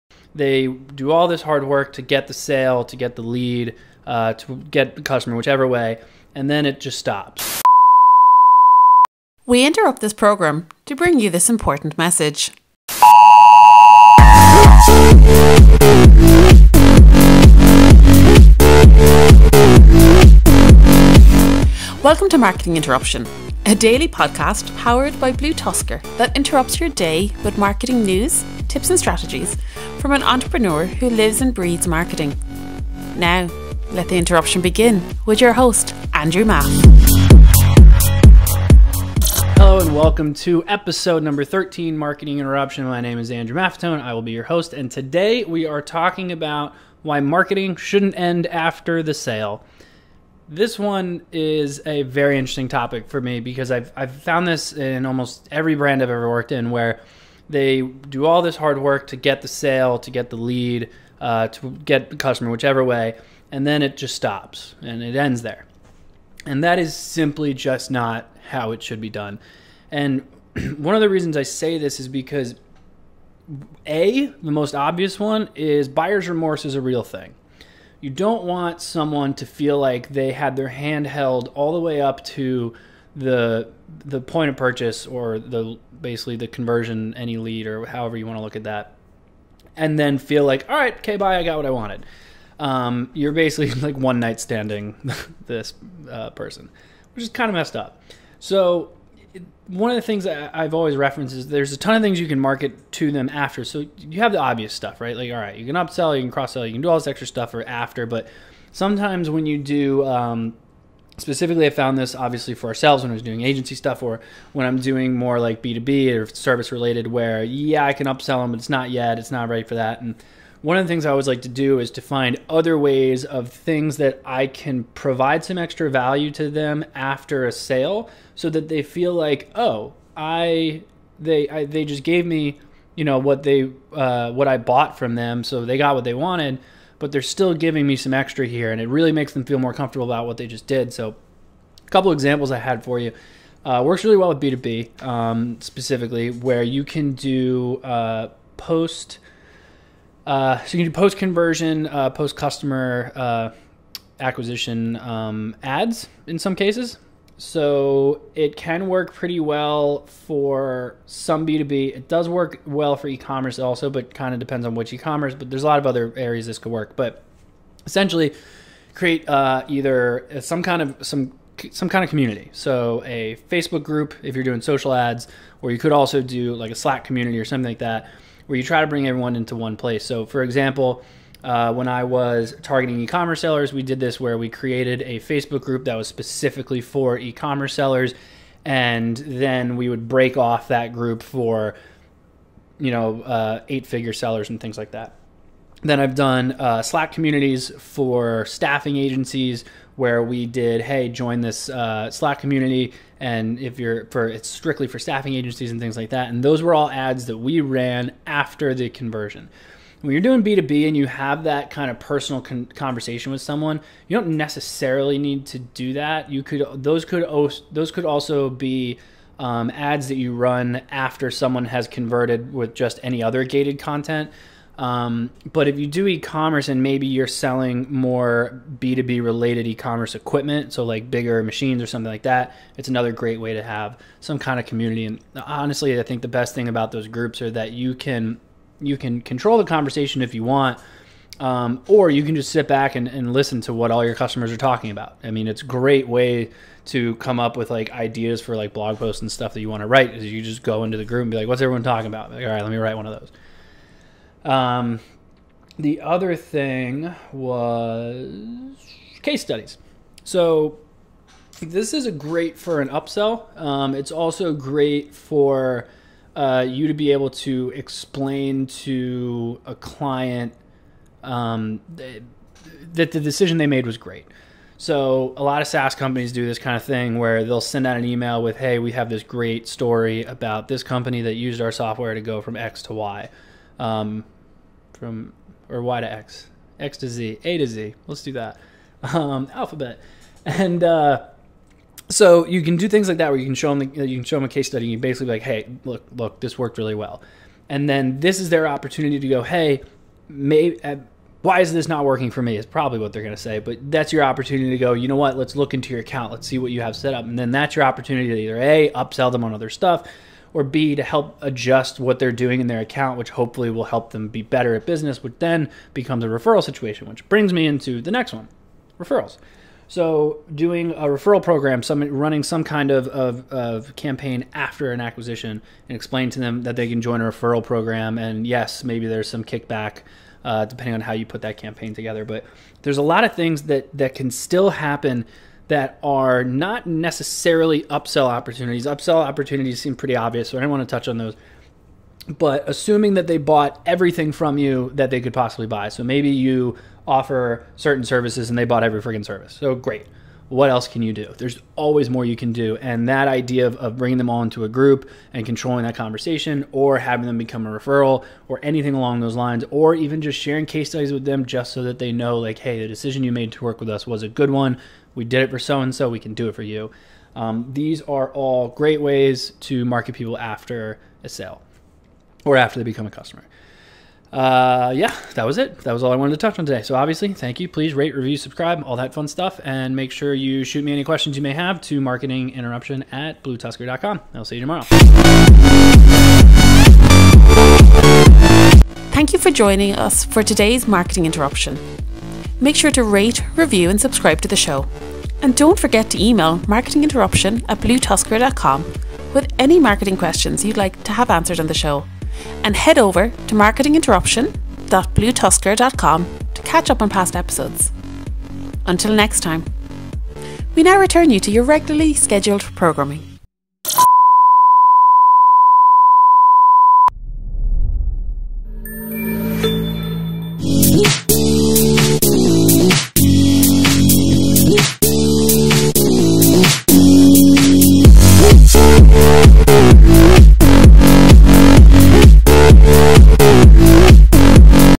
Why Marketing Doesn't End After the Sale | Ep. #13
Welcome to Marketing Interruption a Podcast by BlueTuskr!

On the 13th episode of Marketing Interruption, your host Andrew Maff goes into detail about the theory of continuing marketing after a sale. He talks about even continuing paid advertising after a sale and leveraging a new customer to not only make them feel better about their recent purchase but also to try to bring them all into a community. Listen in as he goes over the many wast to post-conversion market customers and even clients to keep them happy and continue to make money from them.

Tune in and enjoy today's interruption!

Have a marketing question you'd like Andrew to cover in an upcoming episode?

CONNECT:
BlueTuskr.com  |  AndrewMaff.com  |  Twitter: @AndrewMaff